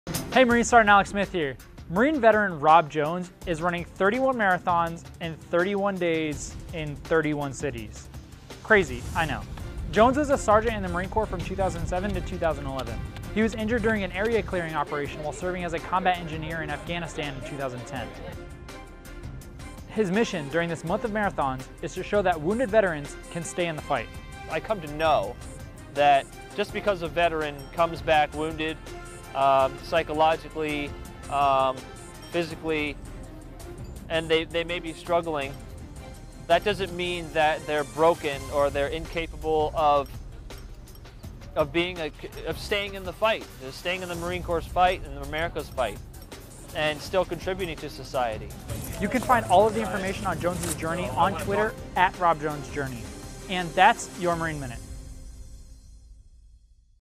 31 Marathons in 31 Days | Veteran Amputee Rob Jones
Double amputee Marine veteran Rob Jones is running 31 marathons in 31 days to show wounded veterans can stay in the fight.